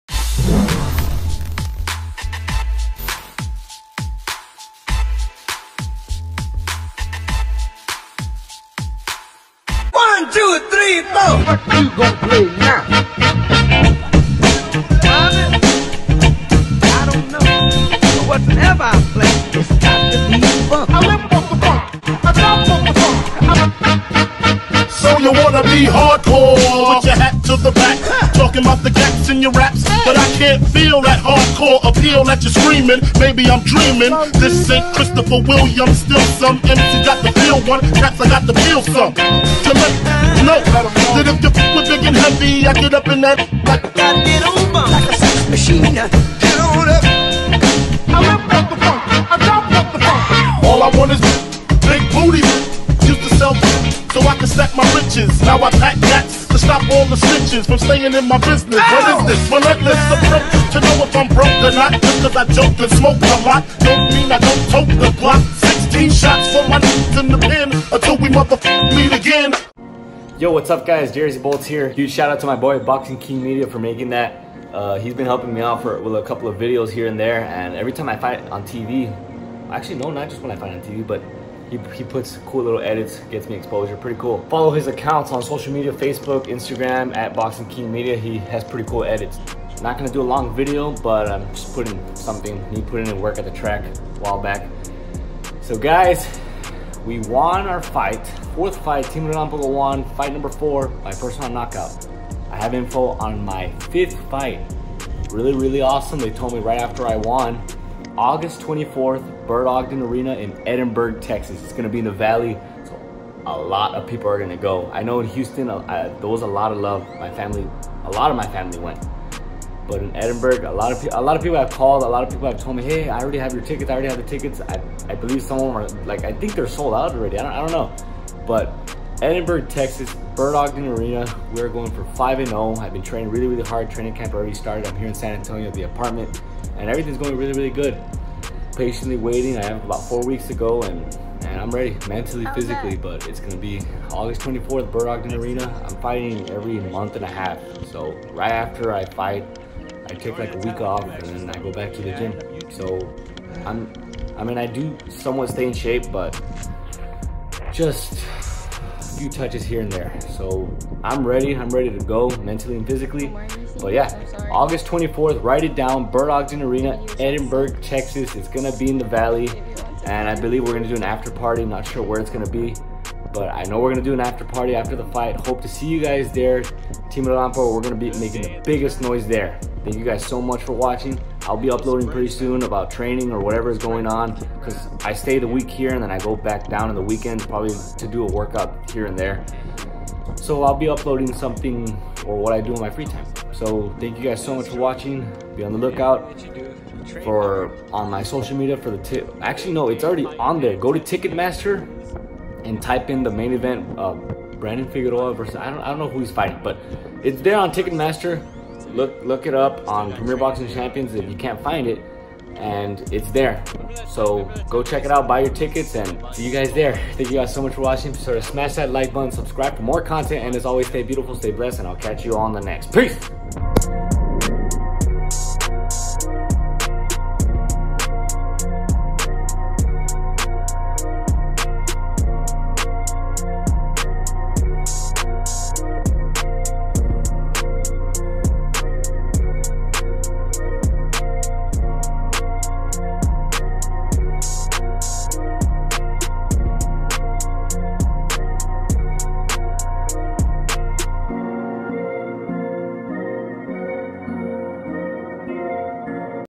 FIGHT #5 ANNOUNCEMENT!!!
Camp has already started, so I’m giving you guys the details about my 5th professional fight!!!